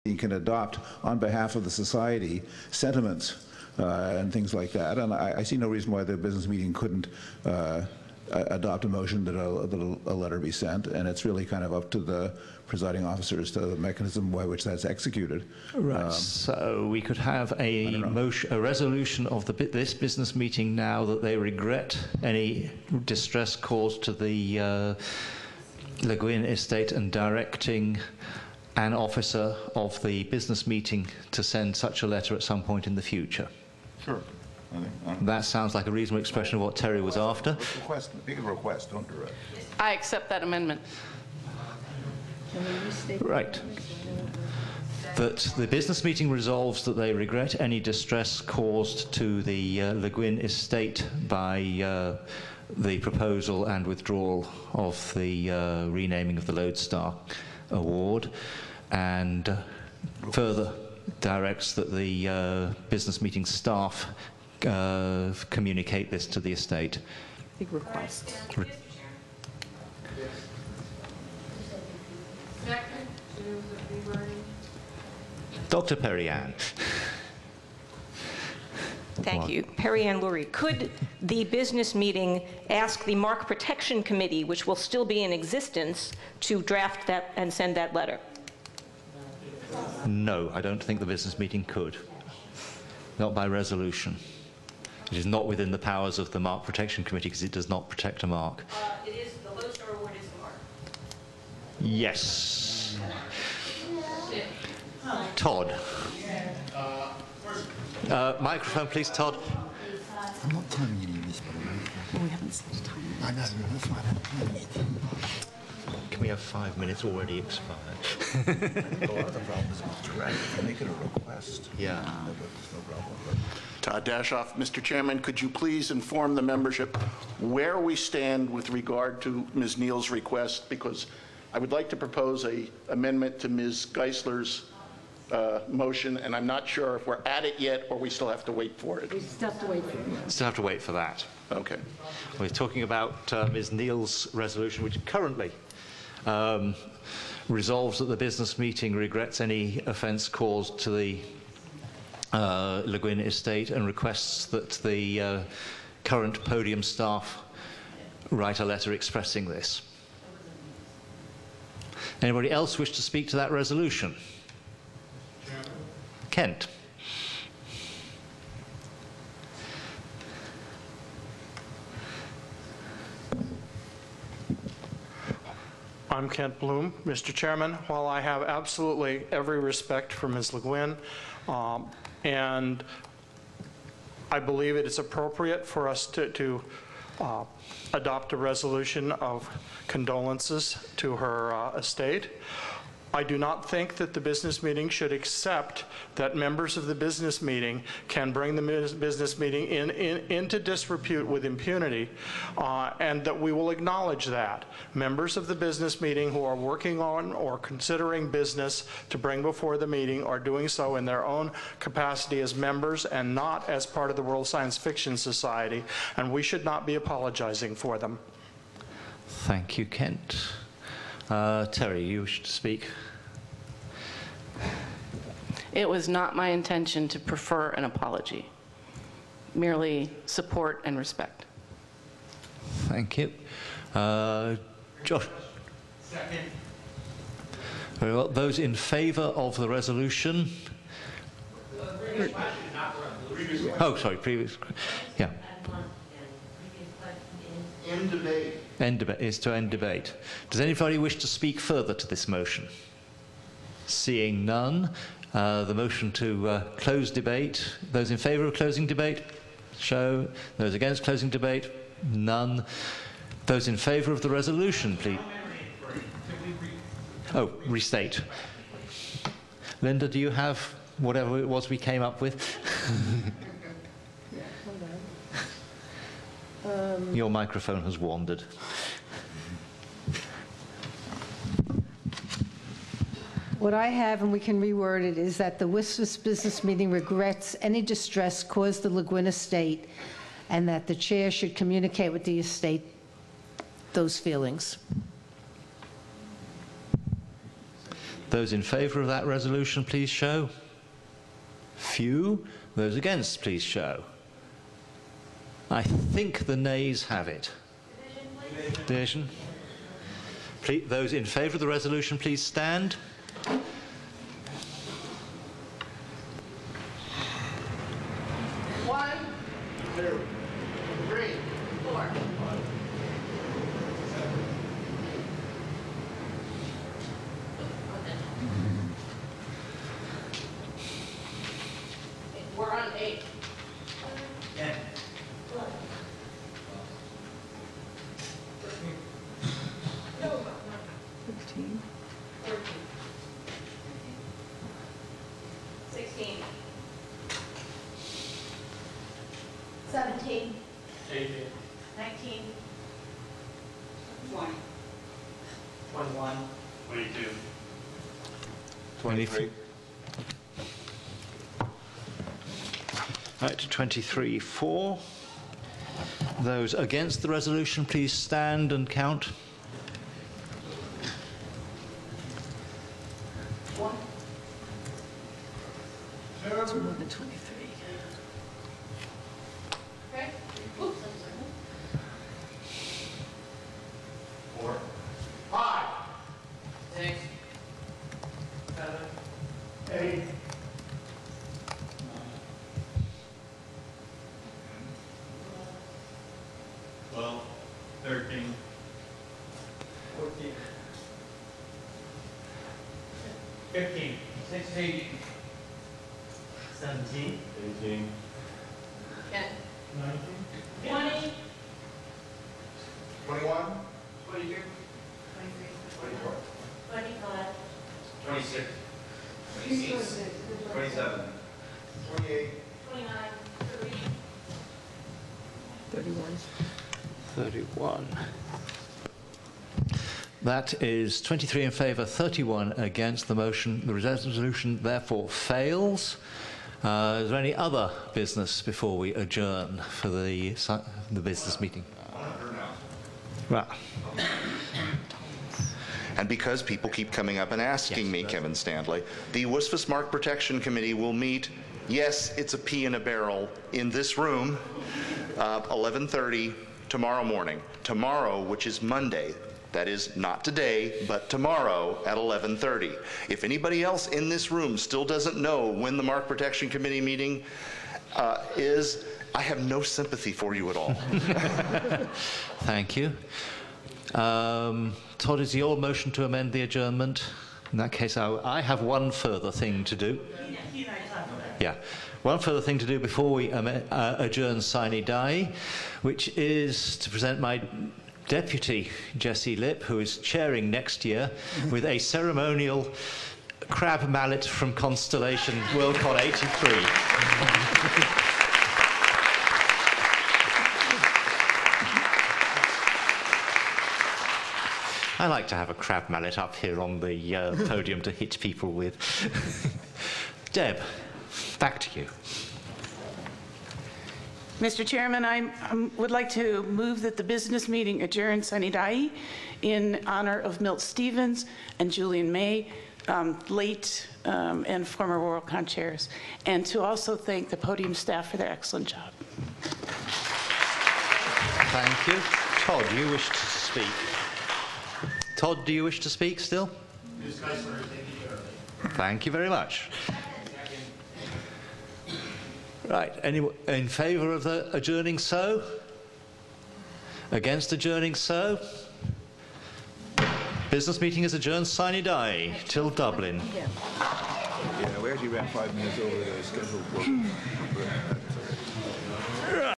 2018 WSFS Business Meeting - Sunday - Part 13
The Business Meeting of the World Science Fiction Society, held at the 76th World Science Fiction Convention, in San Jose, California. The convention ran August 16-20, 2018. Business Meeting sessions started on Friday, August 17.